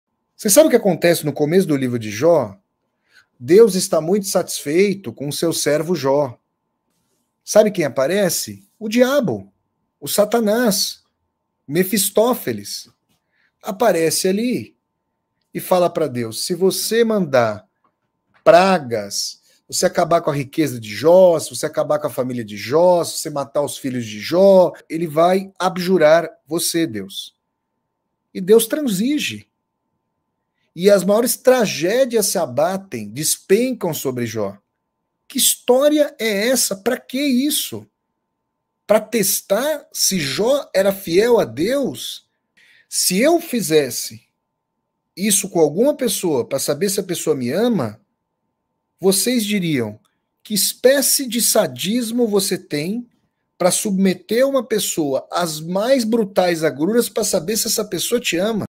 Cortes do Vassoler: Deus e o diabo disputam o destino de Jó
“Cortes do Vassoler: Deus e o diabo disputam o destino de Jó”: fragmento da "Aula com Vassoler: Por que Deus ordenou a Abraão o sacrifício de seu filho Isaac?", ministrada pelo escritor e professor Flávio Ricardo Vassoler em seu canal no YouTube. Link para acesso à aula: